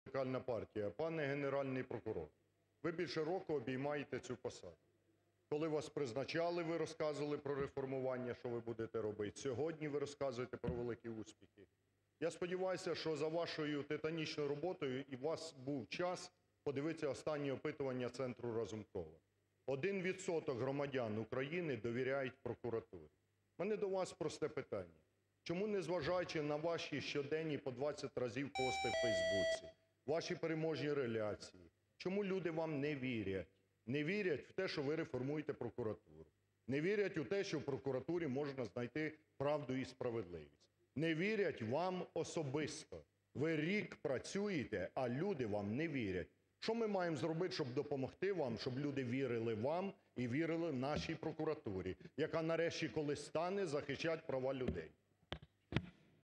Ляшко-Луценку: Чому люди вам не вірять?
Лідер фракції Радикальної партії Олег Ляшко поставив пряме запитання Генпрокурору України Юрію Луценку під час його звіту у парламенті.
«Ви більше року працюєте на посаді Генпрокурора України. Коли вас призначали, ви говорили про те, що будете робити для реформування органів прокуратури, зараз ви розказуєте про «великі» успіхи. Проте, за даними центру Разумкова, лише 1% українців довіряють прокуратурі. Чому, незважаючи на ваші пости по 20 разів на день у Фейсбуці та ваші вихваляння, люди вам не вірять? Не вірять в те, що ви реформуєте прокуратуру, що там можна знайти правду і справедливість, не вірять вам особисто? Що нам потрібно зробити, щоб люди повірили вам та прокуратурі, яка нарешті стане захищати людей?», - звернувся до очільника ГПУ Ляшко.
Проте, на запитання політика Юрій Луценко так і не дав чіткої відповіді.